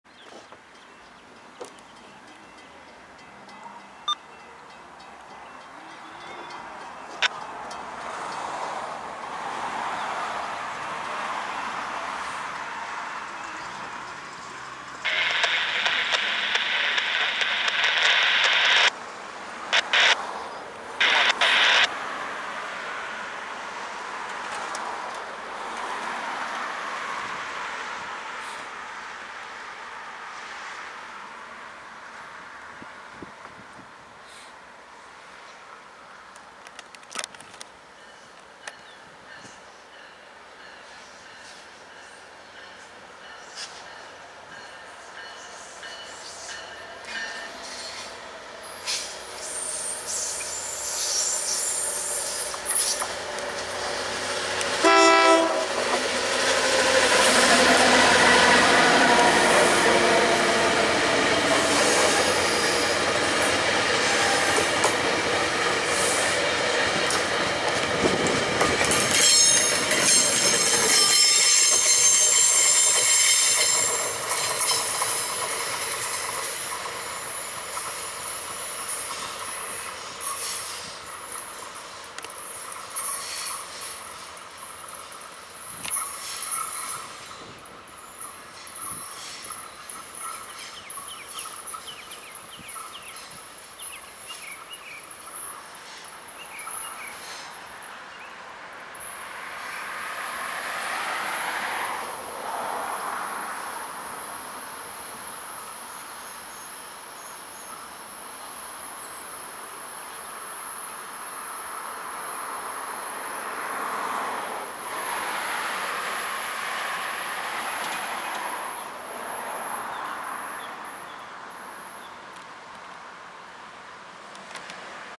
Tri-Rail P669-17 - 9/17/17 - 10:00am - MP SX 969.6
Southbound Tri-Rail Passenger Train with Loco #823 [BL36PH] is pulling four passenger cars.